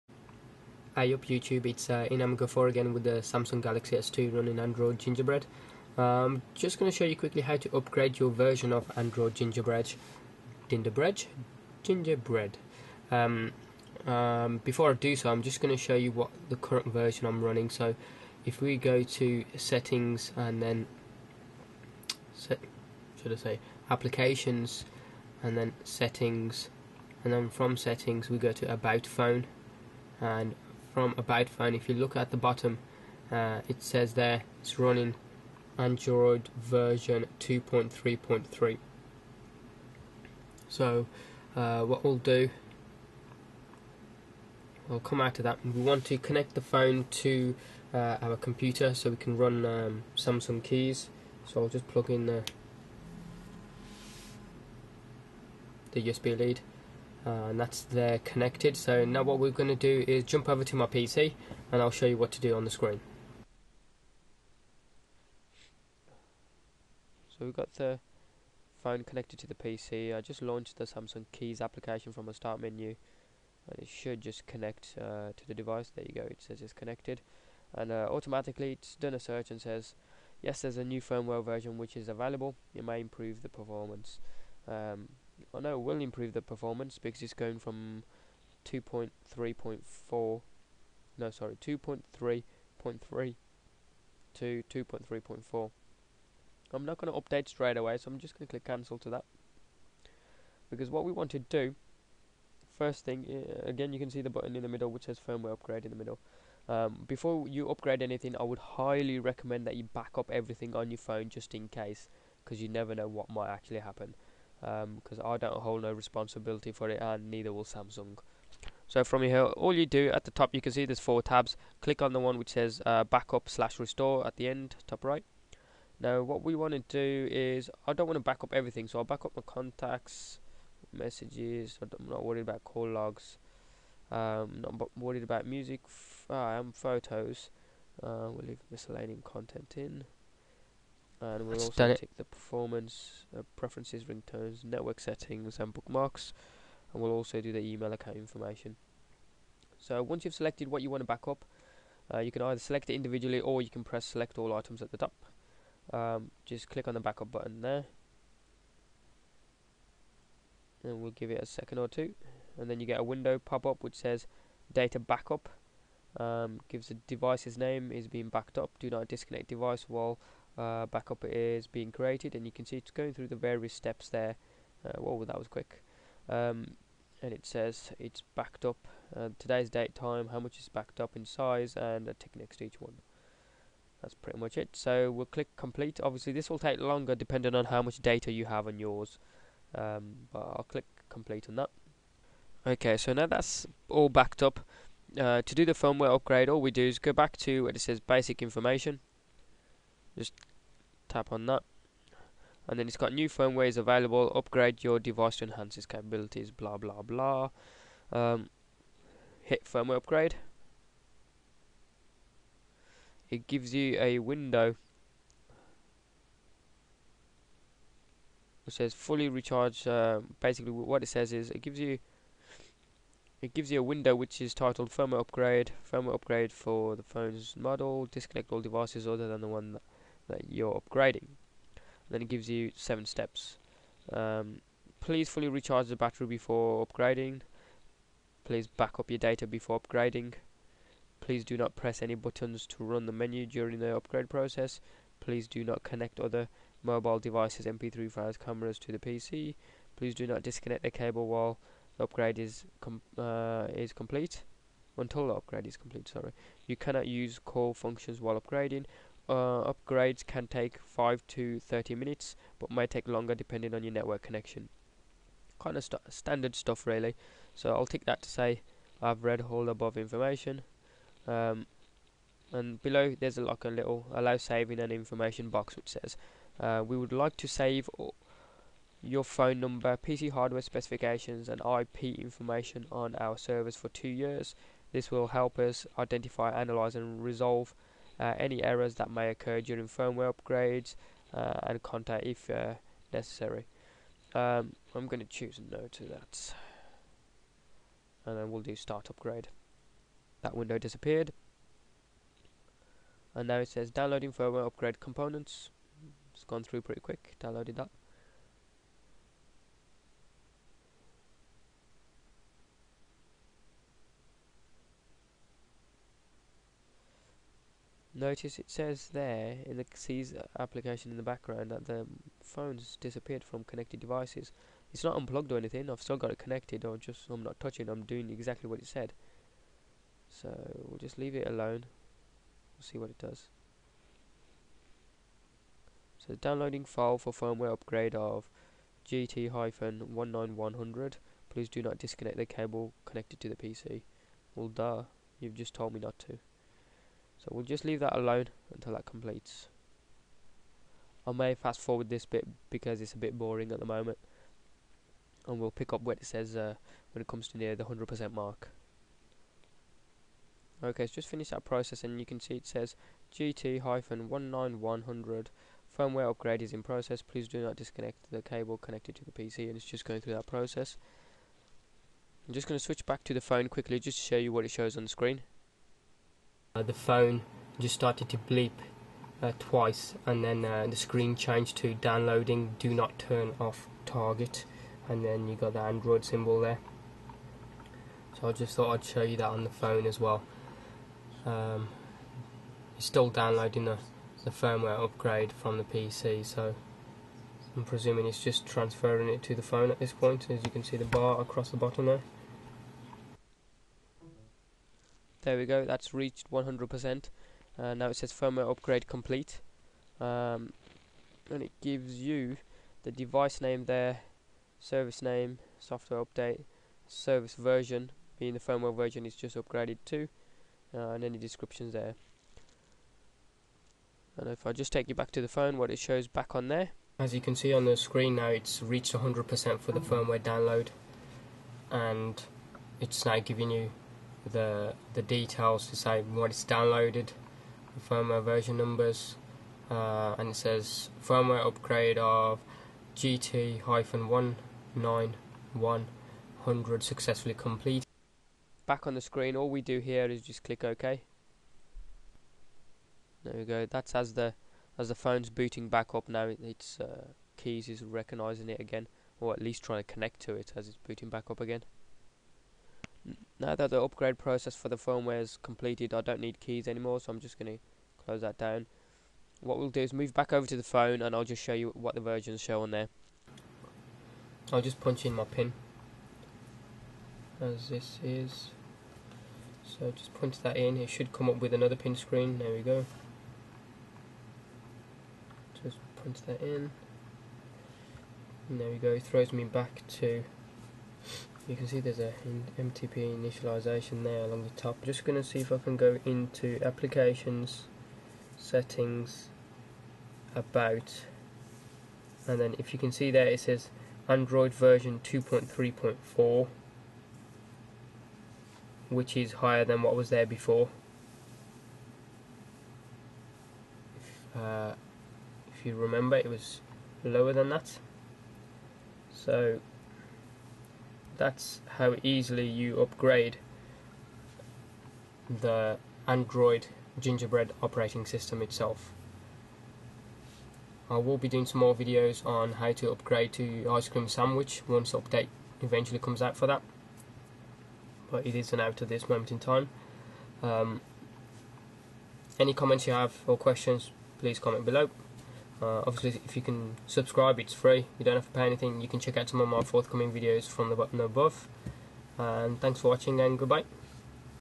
How to Update / Upgrade Android with Samsung Kies App
Just to show you how to update your android operating system the easy way using the Samsung Kies application, in this video its installed on a windows 7 pc. I will be updating from android Gingerbread